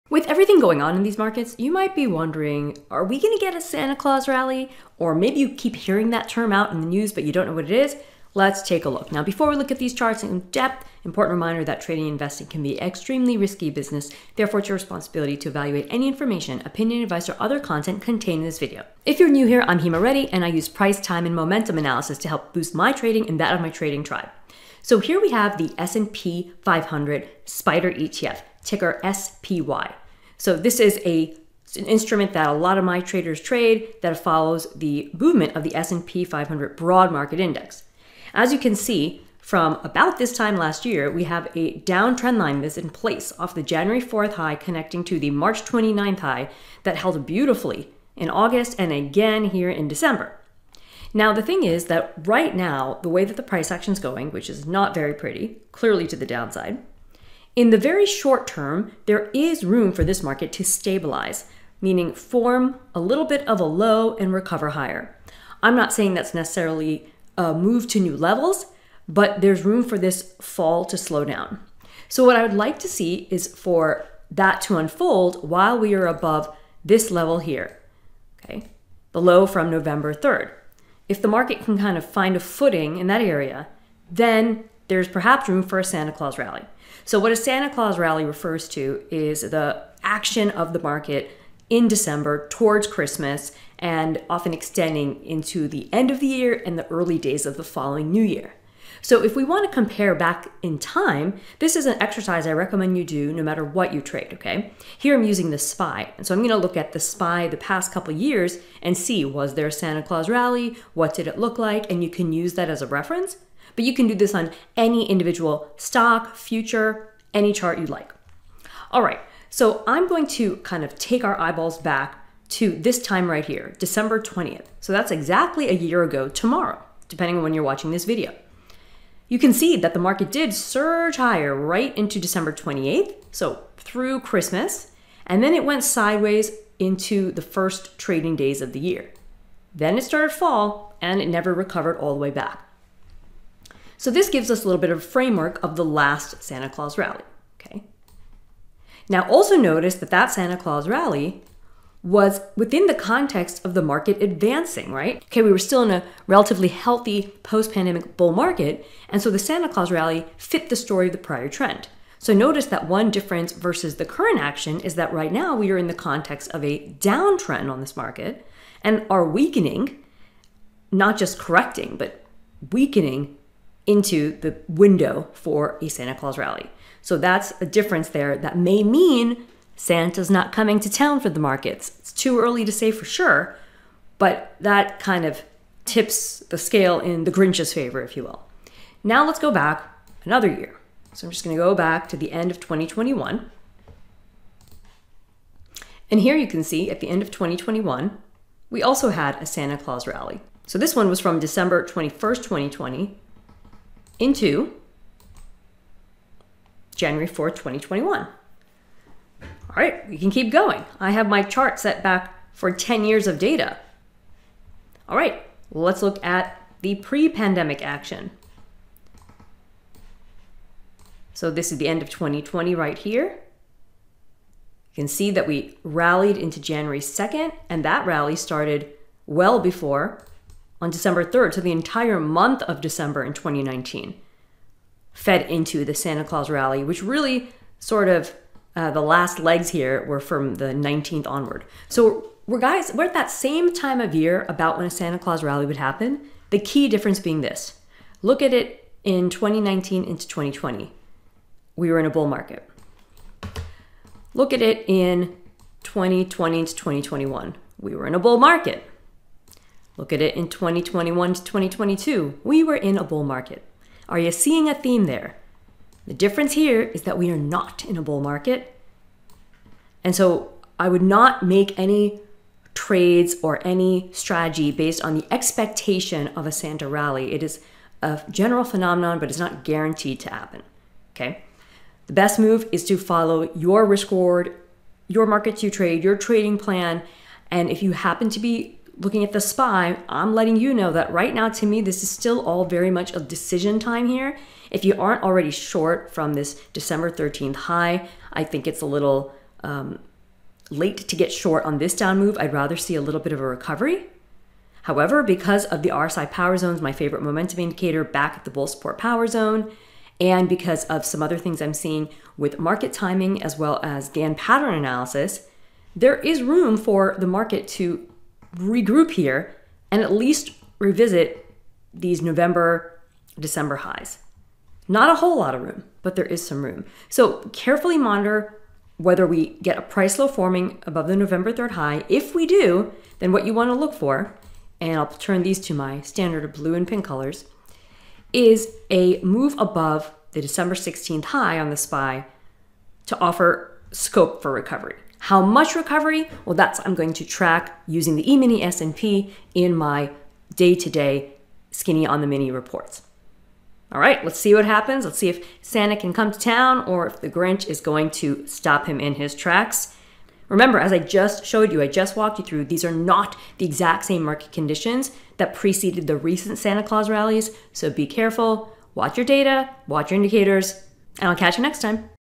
Room for a Santa Claus Rally 2022? Margin is Tiny (Tim)
If you’ve been wondering about whether there’s still room for a Santa Claus rally in these markets, there may be, though the margin is slim (Tiny Tim style)...

Watch the video before you place another trade:
--- Key levels right now on the SPY that will let us know if Santa’s coming to these markets
--- A significant difference between current market conditions and those of recent Santa rallies
--- How to manage your trading between seasonal expectations vs what the charts are saying

To join my Tribe of traders and investors looking to give themselves and their families the life they deserve,